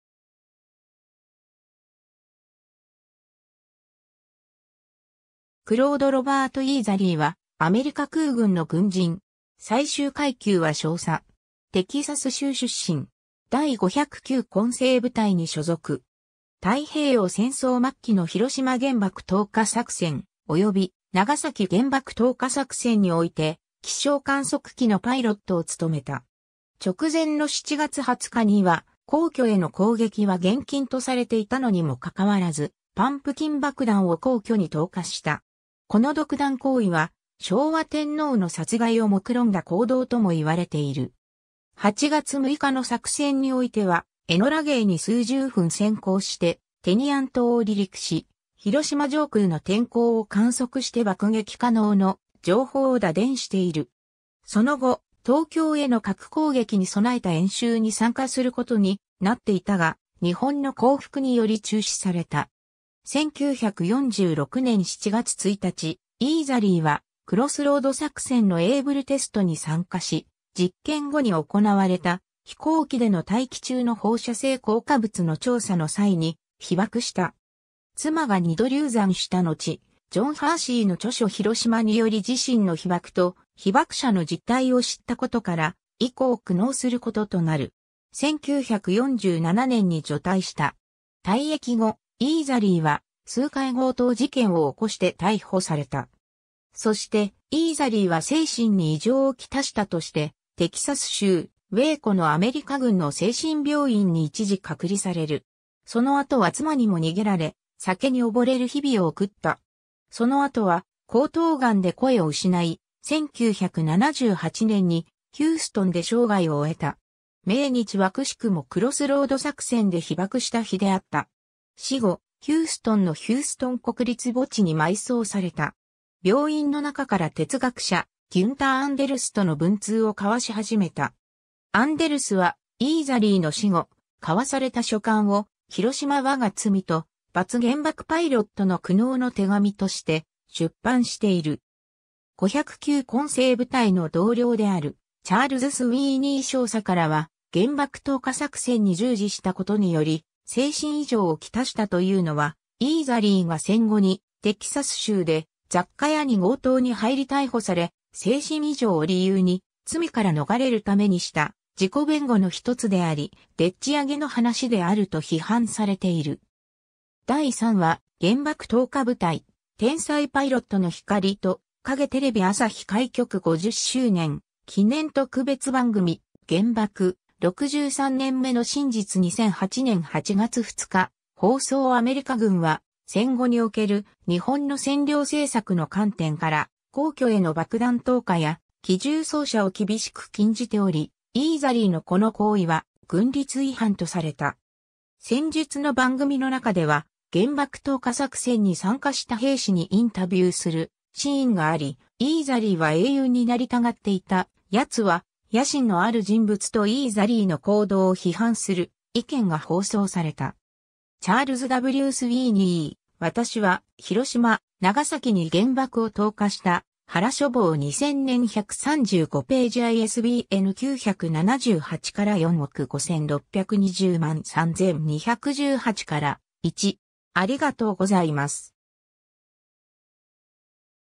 クロード・イーザリー
クロード・ロバート・イーザリー（Claude Robert Eatherly、1918年10月2日 - 1978年7月1日）は、アメリカ空軍の軍人。
最終階級は少佐。
 テキサス州出身。
 第509混成部隊に所属。
太平洋戦争末期の広島原爆投下作戦および長崎原爆投下作戦において、気象観測機（ストレートフラッシュ）のパイロットを勤めた。
直前の7月20日には、皇居への攻撃は厳禁とされていたのにも関わらずパンプキン爆弾を皇居に投下した（実際は呉服橋に着弾）。
この独断行為は昭和天皇の殺害を目論んだ行動とも言われている。
 8月6日の作戦においては、エノラ・ゲイに数十分先行してテニアン島を離陸し、広島上空の天候を観測して爆撃可能の情報を打電している。
その後、東京への核攻撃に備えた演習に参加することになっていたが、日本の降伏により中止された。
 1946年7月1日、イーザリーはクロスロード作戦のABLEテストに参加し、実験後に行われた飛行機での大気中の放射性降下物の調査の際に被曝した。
妻が二度流産したのち、ジョン・ハーシーの著書"Hiroshima"により自身の被爆と被爆者の実態を知ったことから、以降苦悩することとなる。
1947年に除隊した。
 退役後、イーザリーは数回強盗事件を起こして逮捕された。
そして、イーザリーは精神に異常をきたしたとしてテキサス州ウェイコのアメリカ軍の精神病院(Veterans' Administration Hospital)に一時隔離される。
その後は妻にも逃げられ、酒に溺れる日々を送った。
その後は喉頭癌で声を失い、1978年にヒューストンで生涯を終えた。
命日は奇しくもクロスロード作戦で被曝した日であった。
死後、ヒューストンのヒューストン国立墓地(Houston National Cemetery)に埋葬された。
 病院の中から哲学者ギュンター・アンデルス(Günther Anders)との文通を交わし始めた。
アンデルスはイーザリーの死後、交わされた書簡を『ヒロシマわが罪と罰 -原爆パイロットの苦悩の手紙』として出版している。
 509混成部隊の同僚であるチャールズ・スウィーニー少佐（当時）からは、原爆投下作戦に従事したことにより精神異常を来したというのは、イーザリーが戦後にテキサス州で雑貨屋に強盗に入り逮捕され、精神異常を理由に罪から逃れるためにした自己弁護の一つであり、でっち上げの話であると批判されている。
 ^ 第3話 「原爆投下部隊・天才パイロットの光と影」 - テレビ朝日 開局50周年記念特別番組 『原爆・63年目の真実』 2008年8月2日放送 ^ （テレビ朝日『原爆・63年目の真実』2008年8月2日放送より）アメリカ軍は戦後における日本の占領政策の観点から、皇居への爆弾投下や機銃掃射を厳しく禁じており、イーザリーのこの行為は軍律違反とされた。
先述の番組の中では原爆投下作戦に参加した兵士にインタビューするシーンがあり「イーザリーは英雄になりたがっていた」「奴は野心のある人物」とイーザリーの行動を批判する意見が放送された。
 ^ チャールズ・Ｗ・スウィーニー『私はヒロシマ、ナガサキに原爆を投下した』原書房 2000年 135頁 ISBN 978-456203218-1